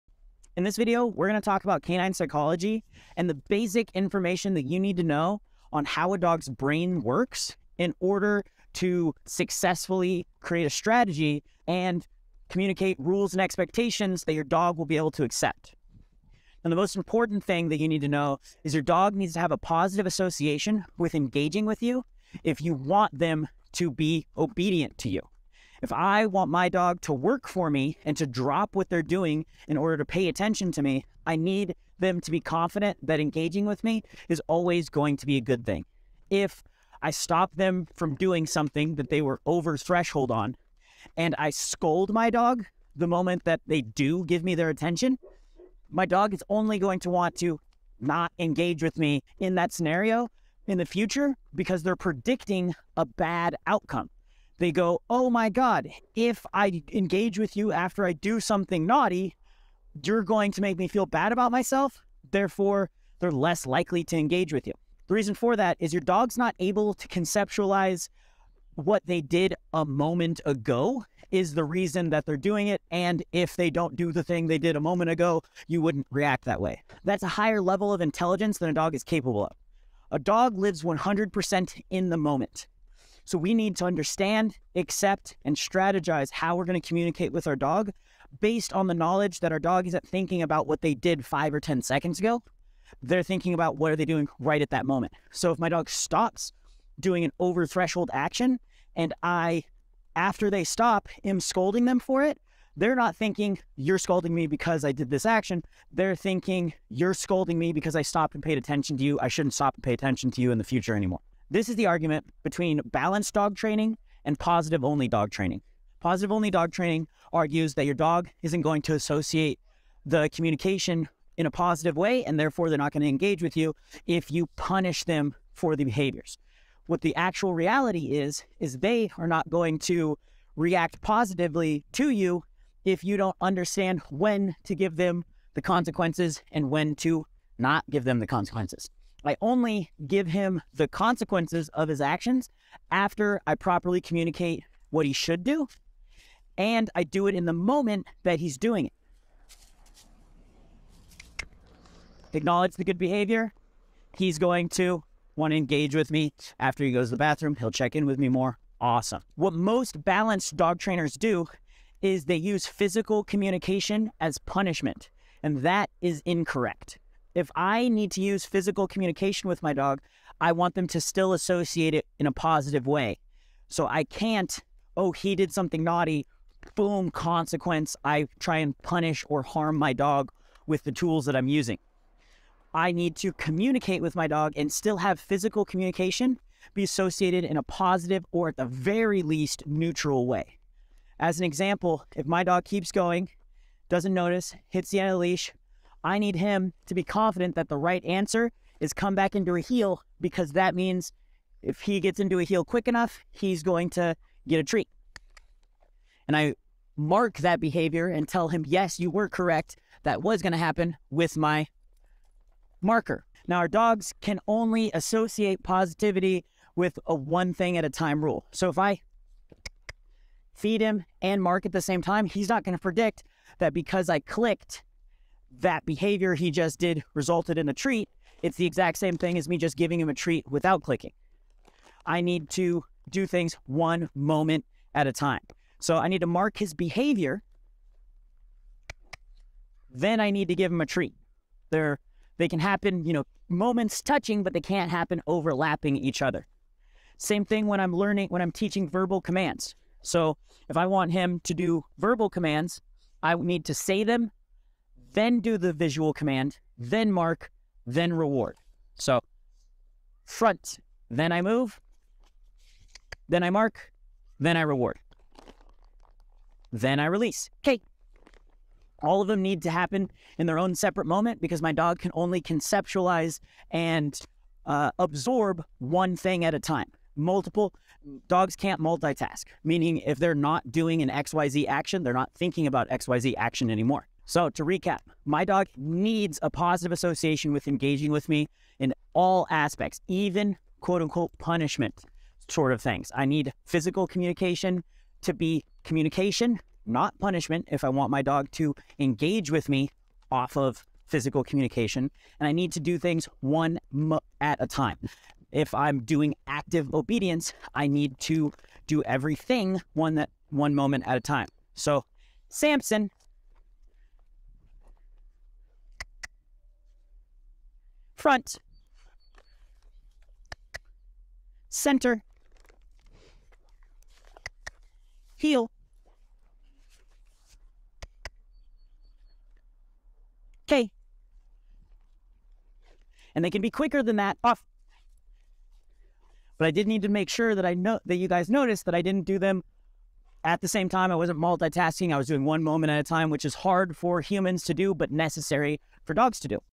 Are You SABOTAGING Your Relationships with Poor Communication?
Learn how to create a positive association with your communication and improve your dog's leash skills with this helpful video on dog training techniques. Whether you're teaching loose leash walking or working on leash skills, these tips will help you and your furry friend communicate effectively and build a strong bond.

0:00 intro
0:18 Positive Association
1:05 Moment
1:51 Balanced vs Force Free
2:47 Physical Communication
3:33 Patterns
4:44 Recap

Private Patron: All funds go towards training abandoned dogs
Abandonmentprevention.com